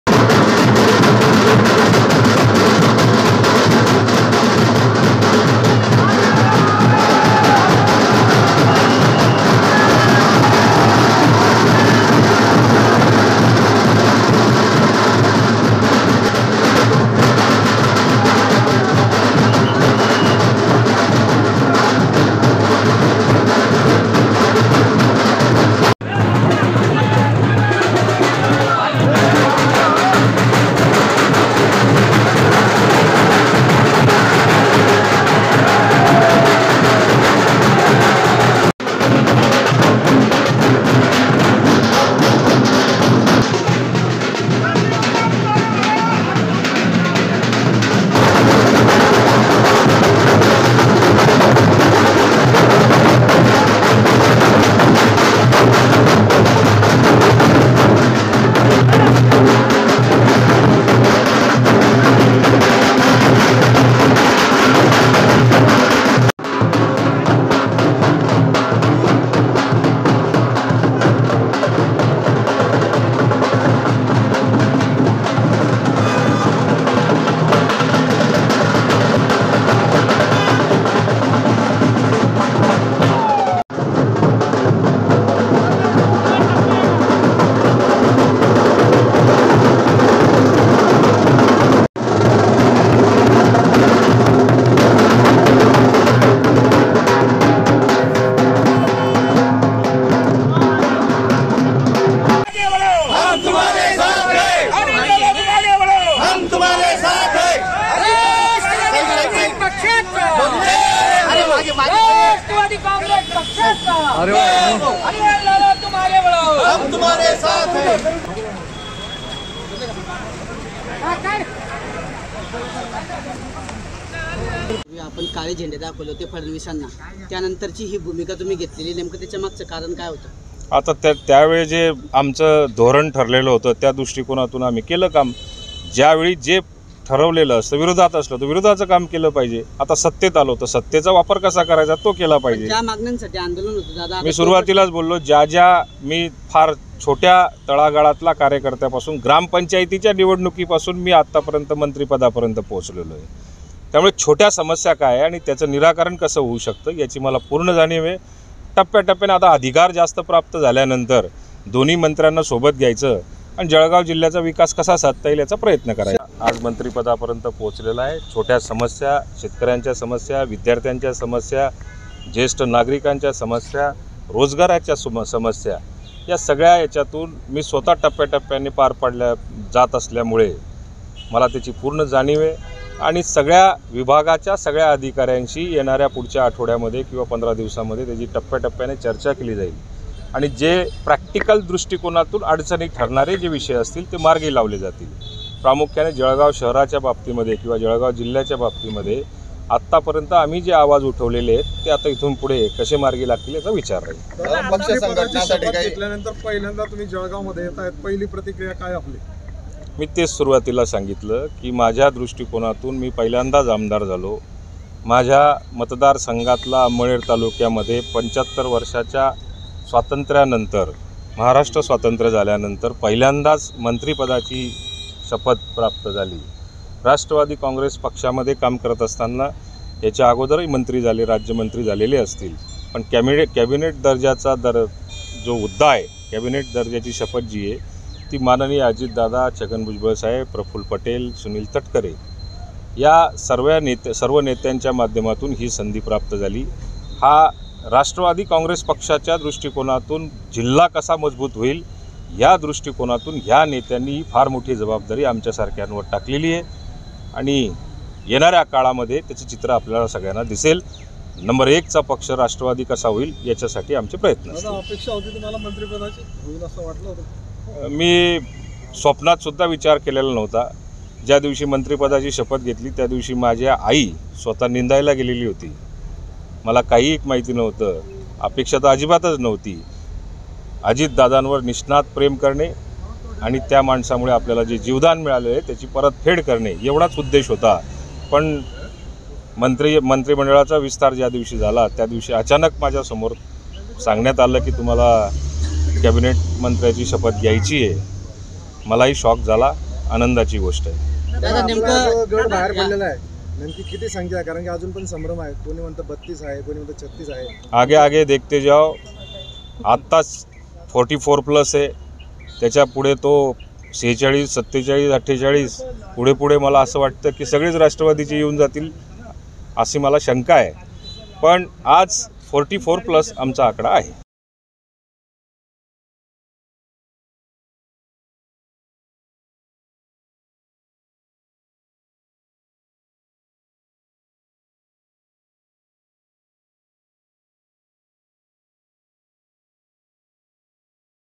MLA Anil Patil | जेसीबीतून फुलांचा वर्षाव... अनिल पाटलांसाठी कार्यकर्ते जोशात | LetsUpp Marathi
अजित पवार यांच्यासोबत मंत्रिपदाची शपथ घेतल्यानंतर राष्ट्रवादी काँग्रेसचे आमदार अनिल भाईदास पाटील यांचे पहिल्यांदाच जळगावात आगमन झाले. यावेळी कार्यकर्त्यांच्या उत्साहाला उधाण आले होते.